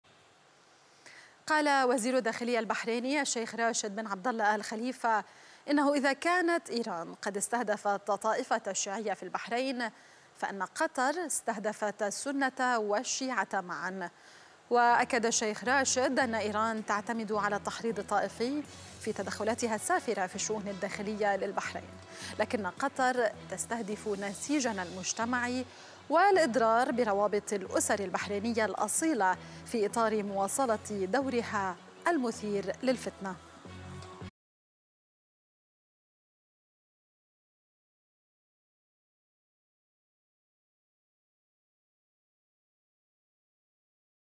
وزير داخلية البحرين: إيران استهدفت الشيعة وقطر تستهدف السنة والشيعة معاً
أخبار MBC